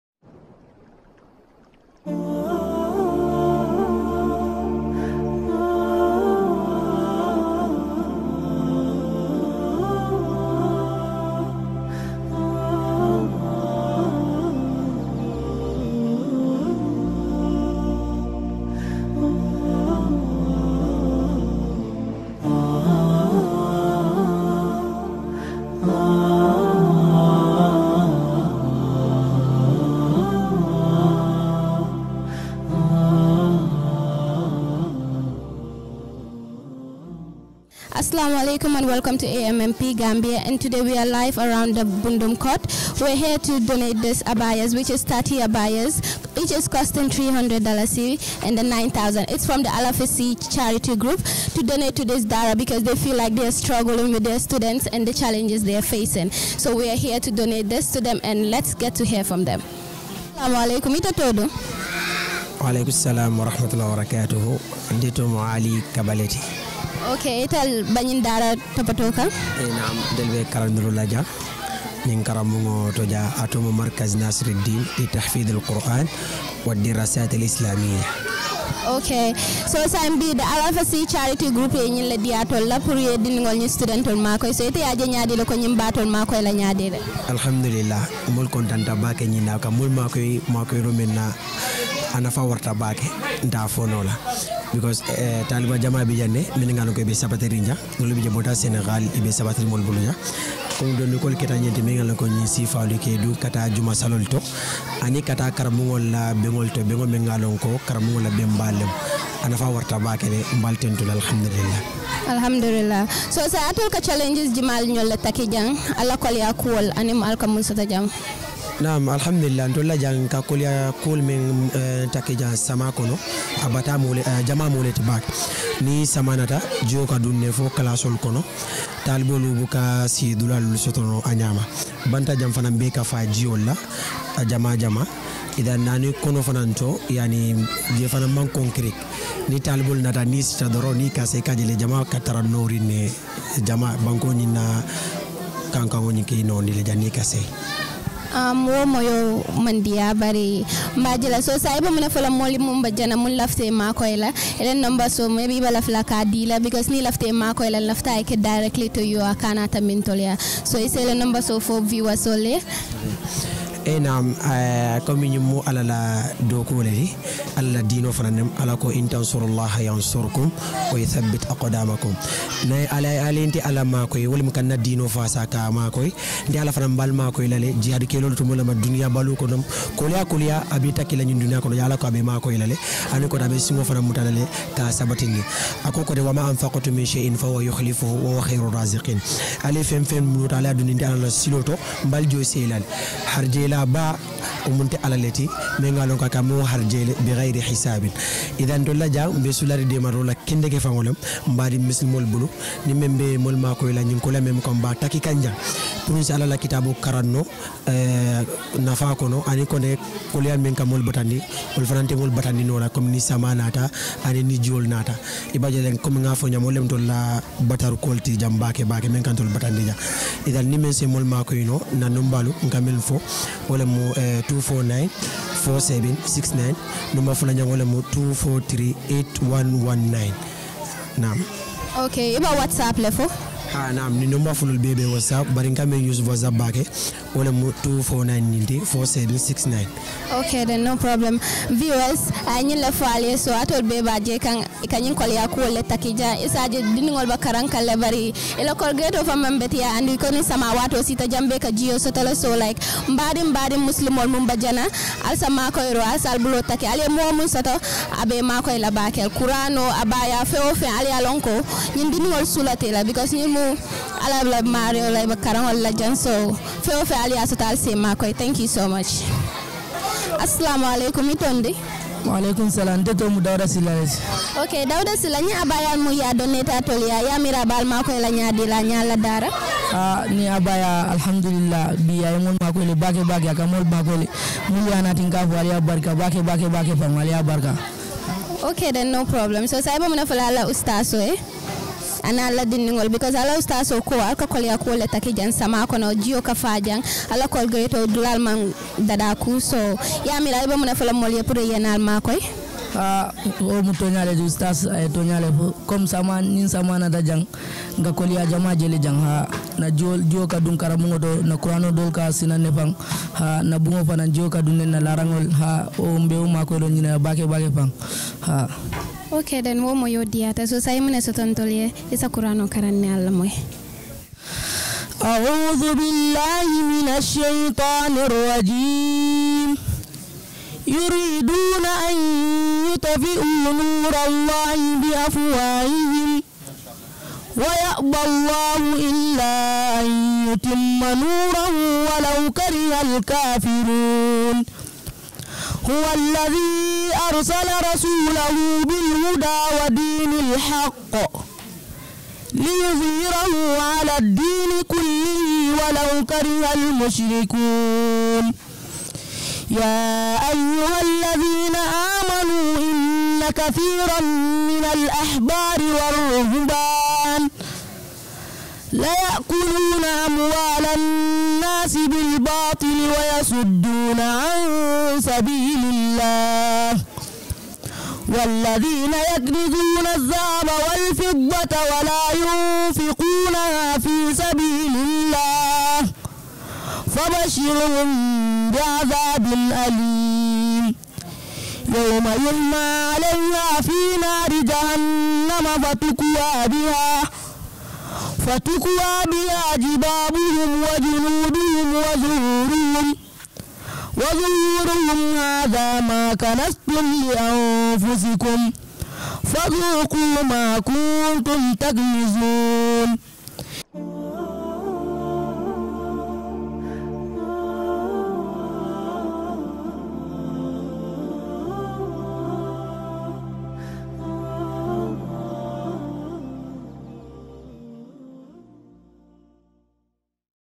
AL-AFASY CHARITY GROUP DISTRIBUTES MEN ABAYAS AMONG THE STUDENTS OF NAASIR DEEN BOARDING SCHOOL
AL-AFASY CHARITY GROUP DISTRIBUTES MEN ABAYAS AMONG THE STUDENTS OF NAASIR DEEN BOARDING SCHOOL-BUNDUNG